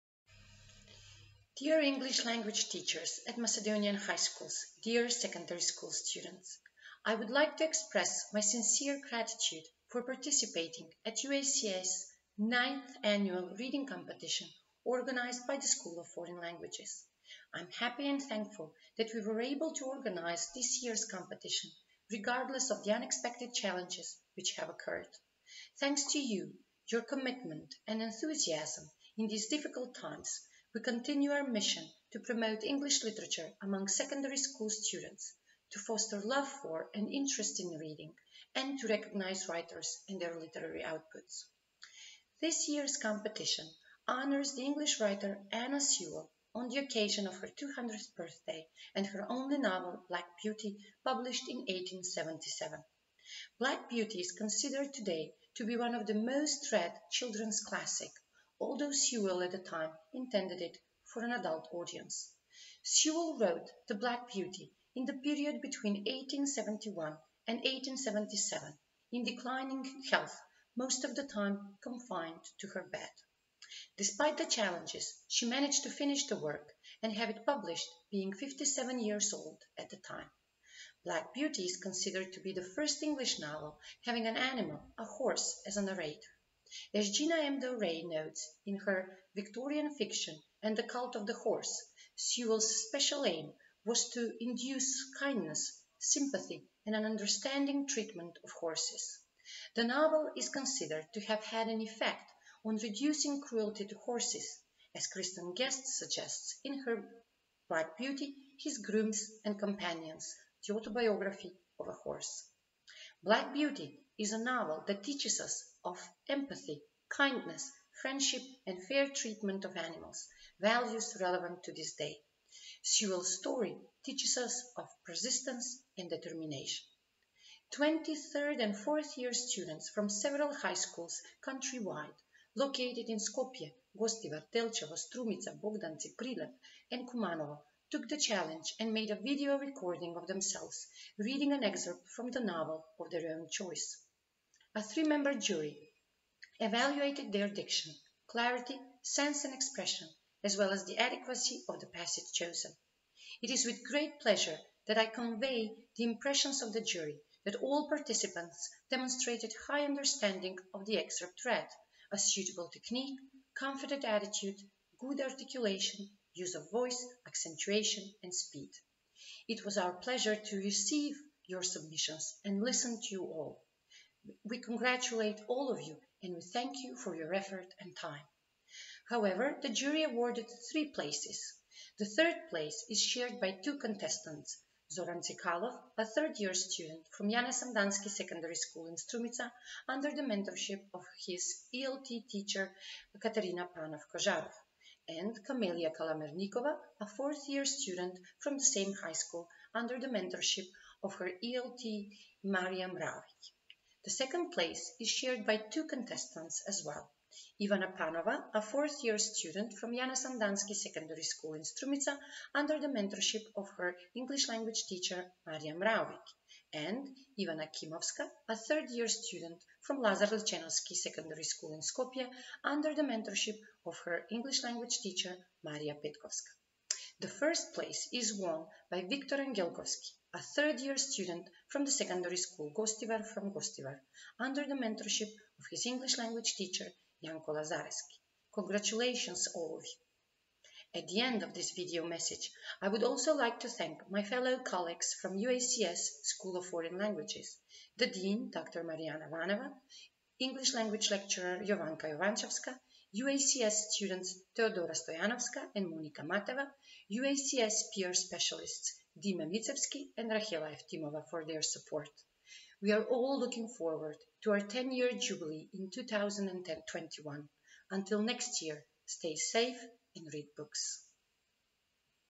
Ivana Trajanoska on UACS Ninth Annual Reading Competition 2020
Ivana Trajanoska, Assistant Professor at University American College Skopje and organizer of UACS Annual Reading Competition